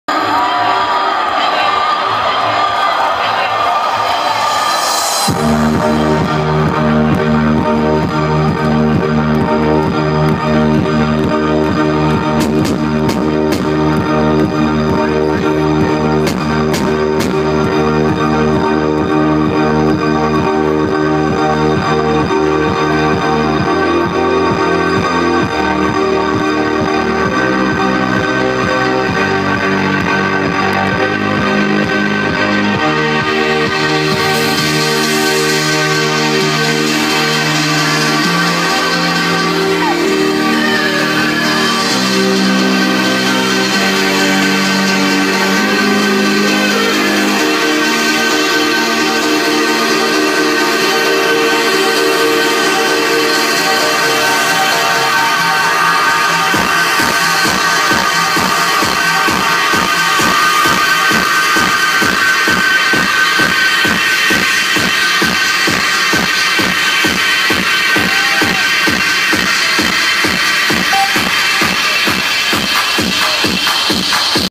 Benny Benassi @Escape From Wonderland San Bernardino, CA
Nos Event Center, San Bernardino

My first day with my iPhone, didn't get the memo about wide video.